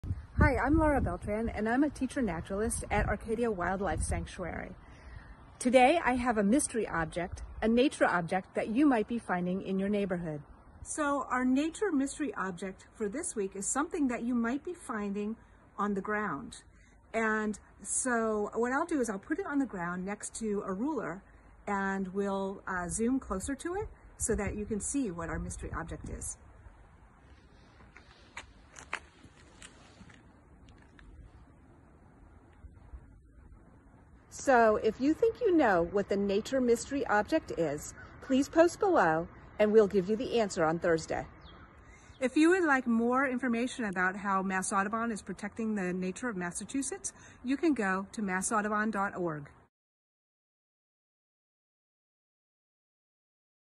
Nature Mystery no. 1 (part 1)
Laura from Mass Audubon's Arcadia Wildlife Sanctuary has a nature mystery. Be sure to watch for part 2.